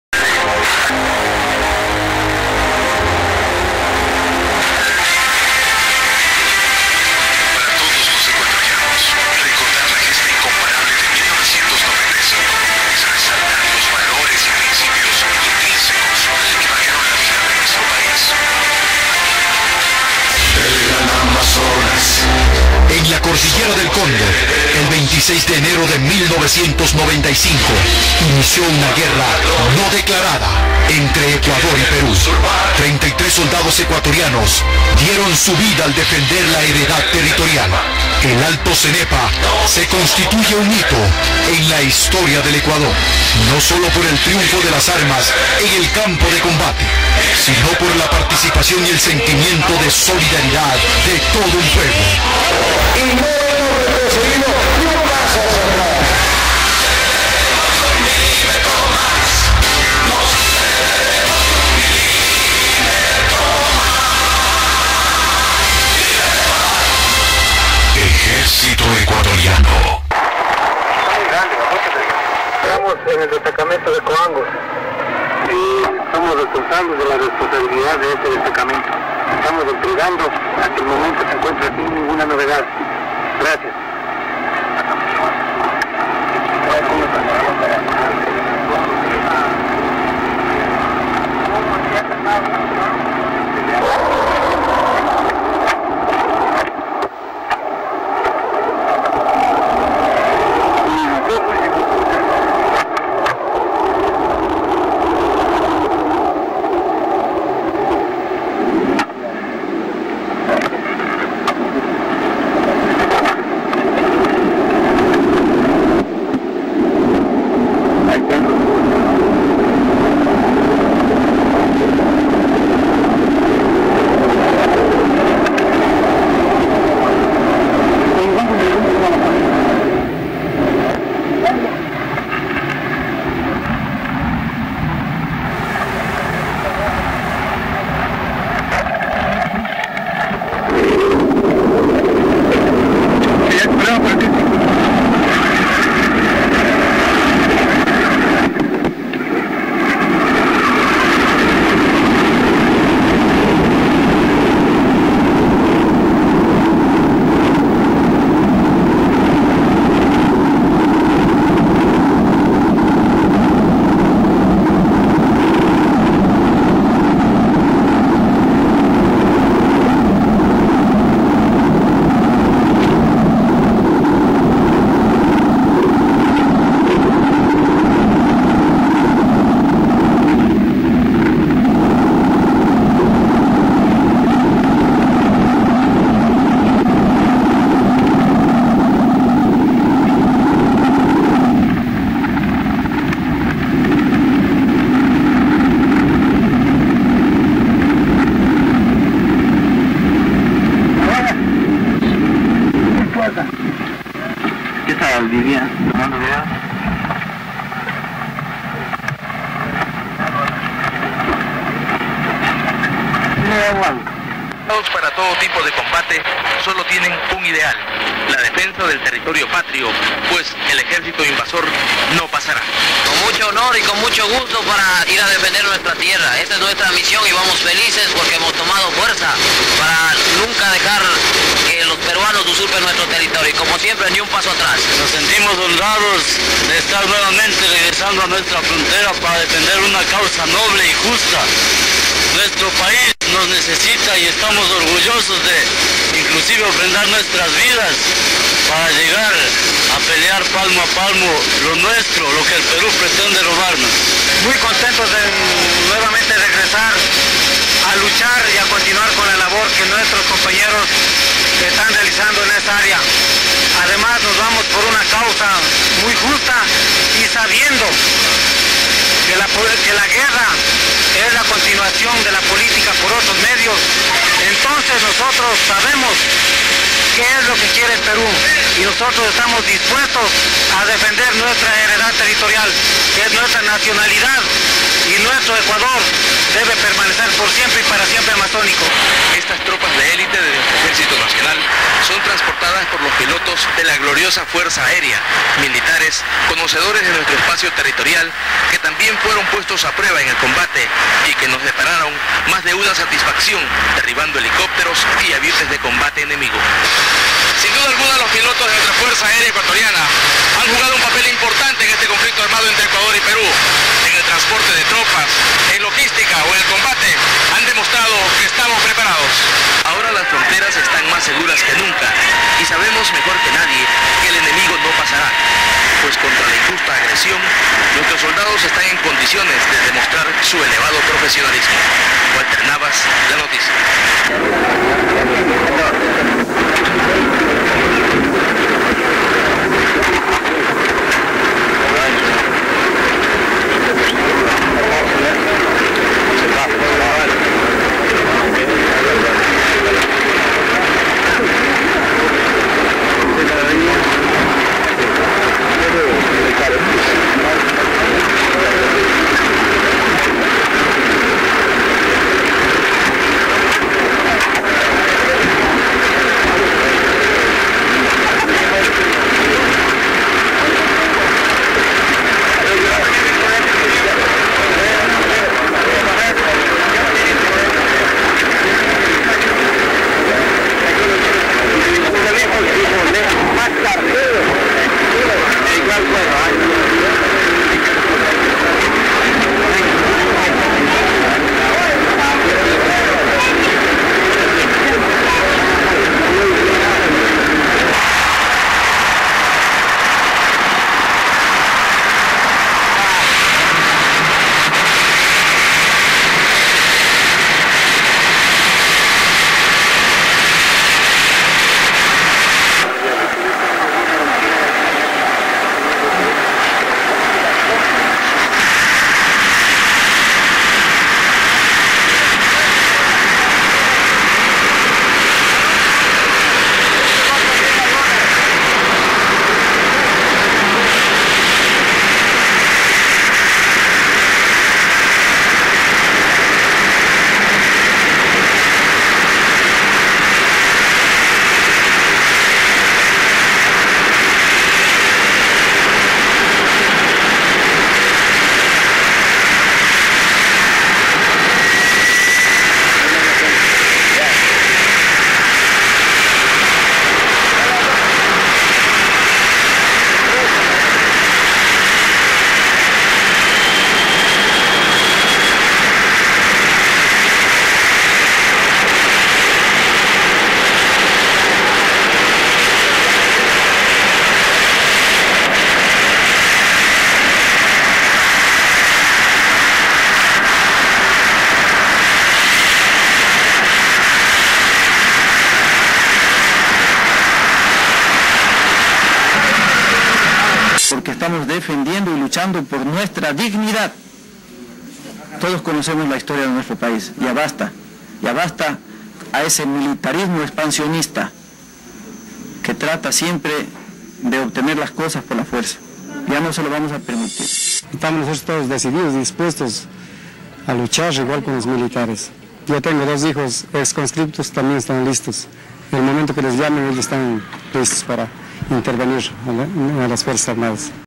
video inedito de la guerra de cenepa
aniversario de la  victoria del cenepa combatientes ecuatorianos rechazan  un  ataque de  aviones  peruanos  supersonicos  sukhoi con misiles antiereos SA iglas en Base  Coangos en febrero 8 de 1995 general josé grijalva comandante de la brigada 21 cóndor  rechaza  agresión peruana aérea en tv